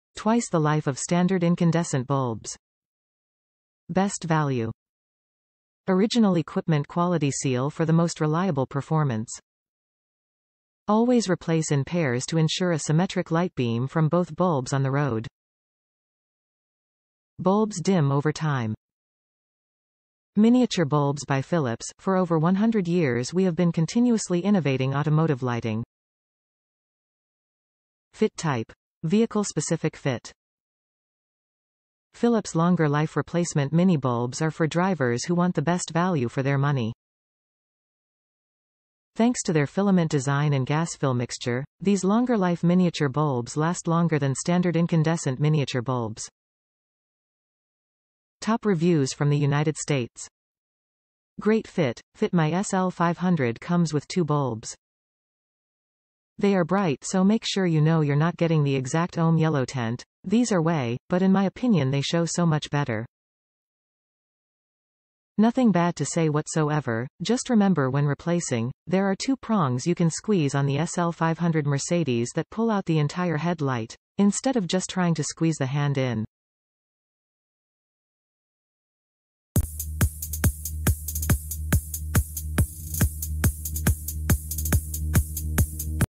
Philips Automotive Lighting P21W LongerLife Miniature Bulb, 2 Pack,White,12498LLB2 - MOI Shop
Philips Automotive Lighting P21W LongerLife Miniature Bulb, 2 Pack,White,12498LLB2

Twice the life of standard incandescent bulbs
Best value
Original equipment quality seal for the most reliable performance
Always replace in pairs to ensure a symmetric light beam from both bulbs on the road. Bulbs dim over time
Miniature bulbs by Philips, for over 100 years we have been continuously innovating automotive lighting
Fit type: vehicle specific fit
Philips LongerLife replacement mini bulbs are for drivers who want the best value for their money. Thanks to their filament design and gas fill mixture, these LongerLife miniature bulbs last longer than standard incandescent miniature bulbs.

Great fit, fit my SL500 comes with two bulbs
-they are bright so make sure you know you’re not getting the exact oem yellow tent, these are way , but in my opinion they show so much better!

Nothing bad to say what so ever, just remember when replacing, there are two prongs you can squeeze on the sl500 Mercedes that pull out the entire head light, instead of just trying to squeeze the hand in.